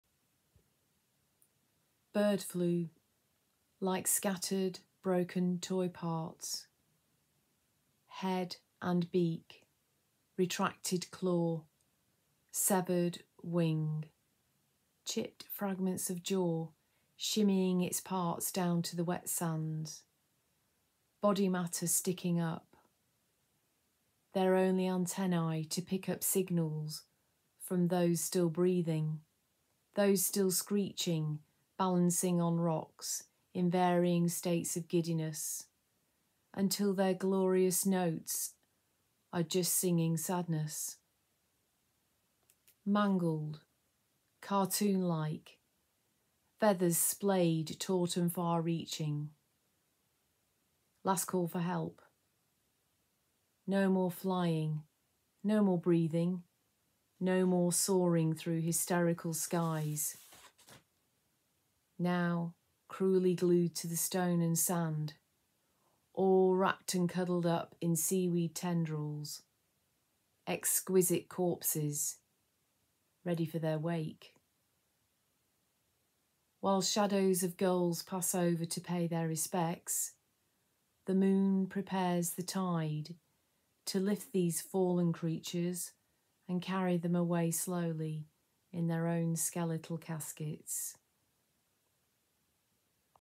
Bird Flu by Anne Cartwright
Poet’s Biography
I was born an identical twin in 1965 in Bolton England. I studied Fine Art Painting. From the age of seven I was surrounded by sea and skies, always welding with nature. My painting informs my writing. My writing informs my painting. Both describe the world around me and the imagined. Writing in situ invites the extraordinary and the unexpected. Well it’s all around us, rich pickings. . . .look no further than the seemingly obvious.

At first I thought it was the mighty force of the sea. Looking down at this repeating horror, bevelled out bodies, hollow as empty soup bowls, birds tossed like feathered toys. I realised that all these beautiful black headed gulls were victim to Avain Flu. I visited the beach repeatedly, wrote on the cliff top looking down and up close. This poem is snap shots and how I imagined the dead birds communicating with the living birds.

Poem Text
Like scattered broken toy parts
Head and beak
Retracted claw
Severed Wing
Chipped fragments of jaw
Shimmying it’s parts down to the wet sands
Body matter sticking up
Their only antenna
to pick up signals
from those still breathing
those still screeching
balancing on rocks
In varying states of giddiness
Until their glorious notes
are just singing sadness

Mangled
Cartoon-Like
Feathers splayed, taught and far-reaching
LAST CALL FOR HELP
No more flying
No more breathing
No more soaring through hysterical skies

Now cruelly glued to stones and sand
All wrapped and cuddled in sea seaweed tendrils
EXQUISITE CORPSES
Ready for their wake

While shadows of gulls pass over to pay their respects
The moon prepares the tide
to lift these fallen creatures
And carry them away slowly
In their own skeletal caskets